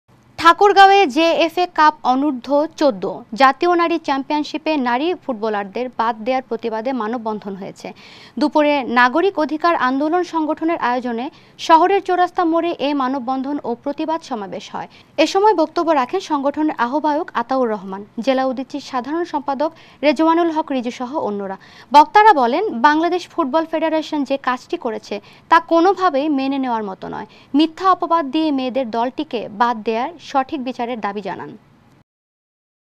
ঠাকুরগাঁওয়ে বাফুফে’র বিরুদ্ধে মানববন্ধন
ঠাকুরগাঁওয়ে ‘জেএফএ কাপ অনুর্ধ-১৪’ জাতীয় নারী চ্যাম্পিয়নশীপে নারী ফুটবলারদের বাদ দেয়ার প্রতিবাদে মানববন্ধন হয়েছে।
দুপুরে ‘নাগরিক অধিকার আন্দোলন’ সংগঠনের আয়োজনে শহরের চৌরাস্তা মোড়ে এ মানববন্ধন ও প্রতিবাদ সমাবেশ হয়। এসময় বক্তব্য রাখেন, সংগঠনের আহ্বায়ক আতাউর রহমান, জেলা উদীচীর সাধারণ সম্পাদক রেজওয়ানুল হক রিজুসহ অন্যরা। বক্তারা বলেন, ‘বাংলাদেশ ফুটবল ফেডারেশন’ যে কাজটি করেছে তা কোনো ভাবেই মেনে নেয়ার মতো নয়। মিথ্যা অপবাদ দিয়ে মেয়েদের দলটিকে বাদ দেয়ায় এর সঠিক বিচারের দাবি জানান।